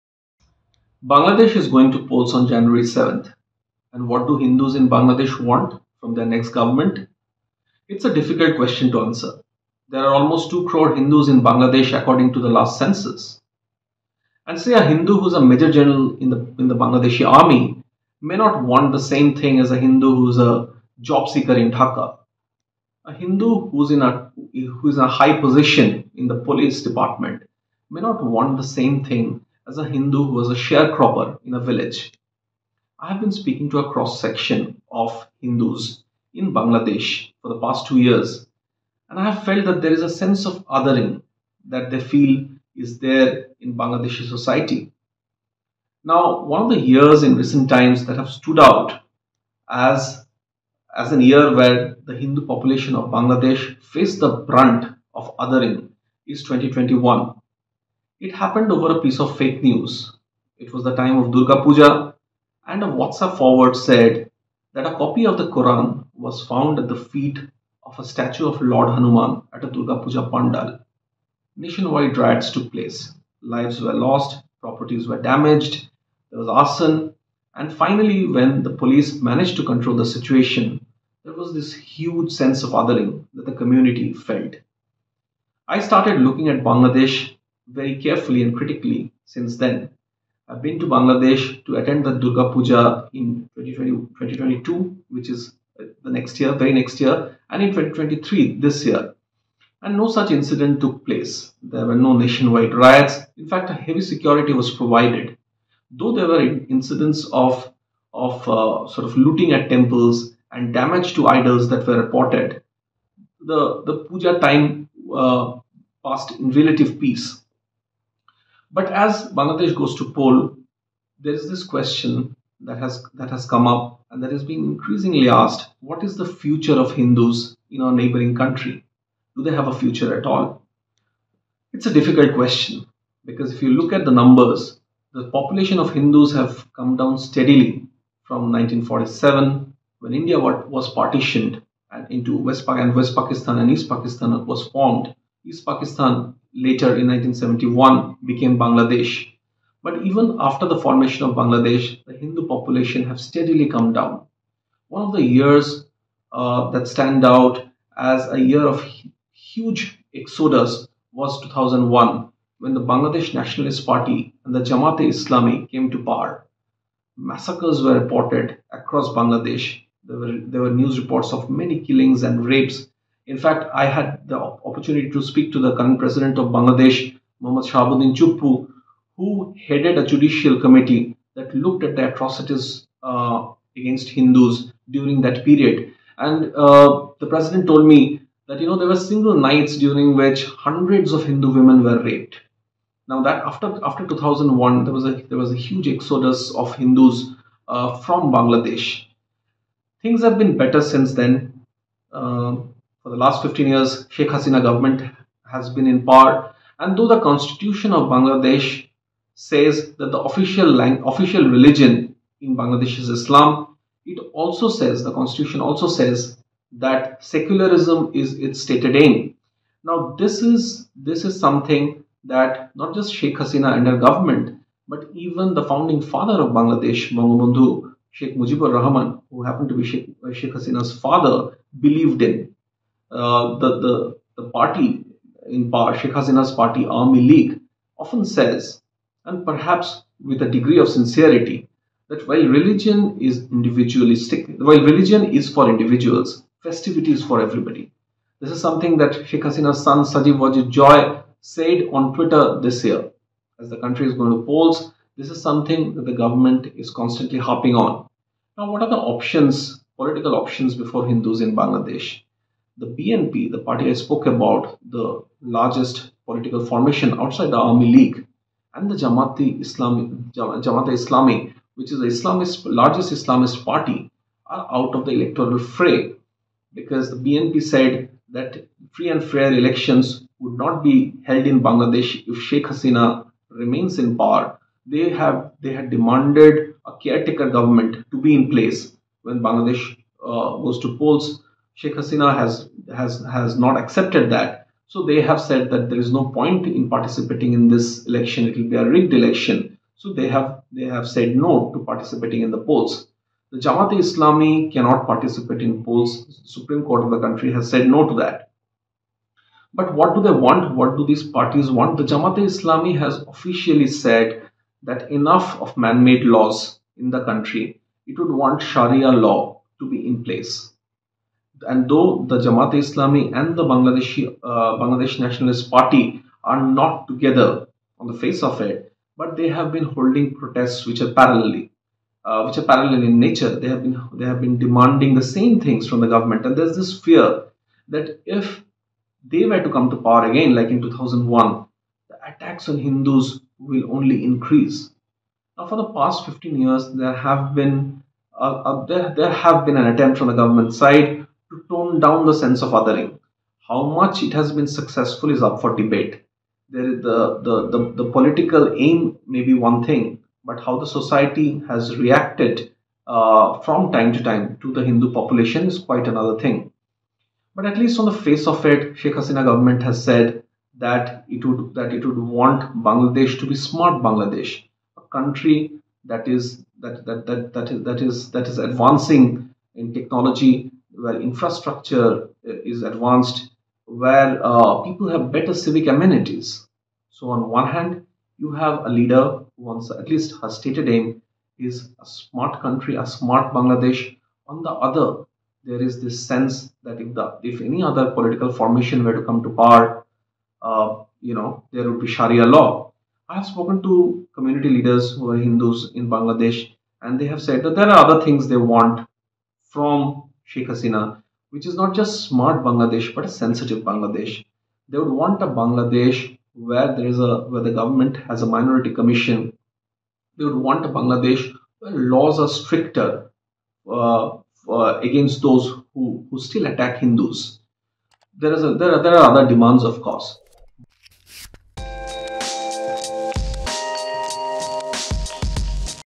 As Bangladesh goes to polls on 7 January 2024, what do Hindus want?
Bangladesh has almost 2 crore Hindus, accounting for 7.9 per cent of the country's population. There have been reports  of attacks on Hindus from time to time in recent years. The Sheikh Hasina government has been in power for 15 years and there is a sense of othering that Hindus feel in Bangladeshi society. But the other political alternative may be far worse with the imposition of Sharia law in Bangladesh. Journalist and Author Deep Halder writes